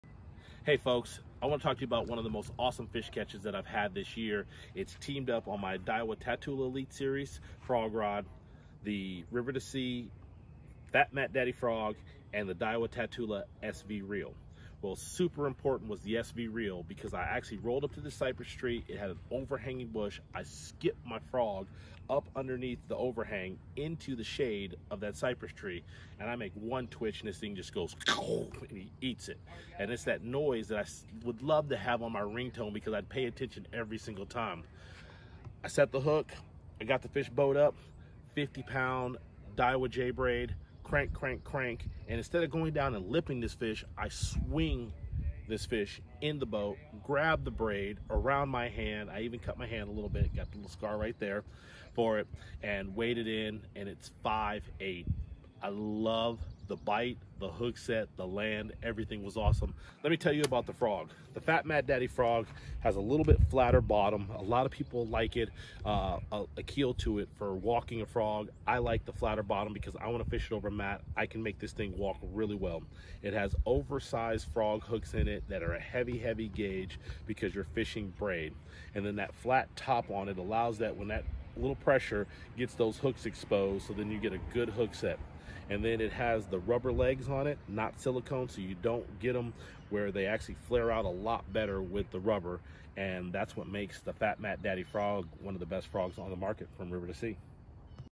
Epic Frog Fish!
If you are into frog fishing you're gonna love this one! Take a look at one of Ish Monroe's most memorable catches so far this year!

Keep up to date with everything going on in the fishing world

Geat all the tackle you need to FISH LIKE ISH here!